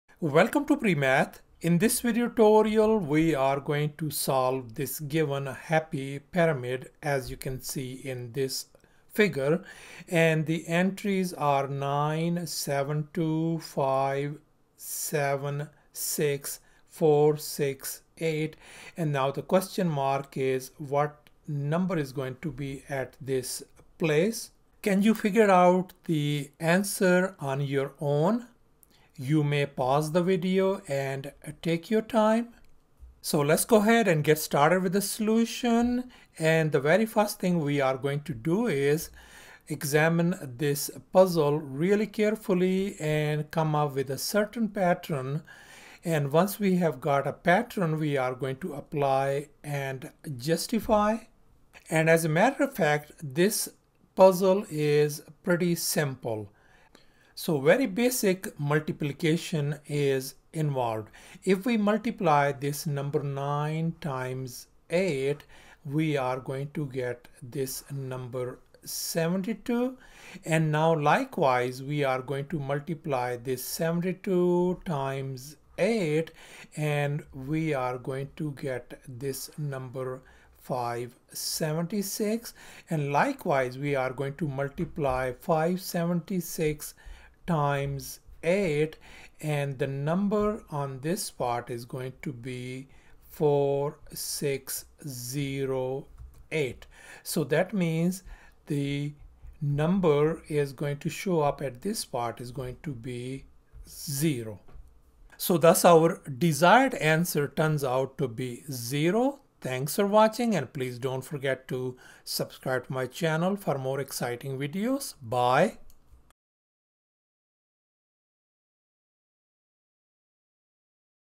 Solve this Happy Pyramid | Fun Puzzle!
Can you solve this happy pyramid math puzzle. Easy explanation by PreMath.com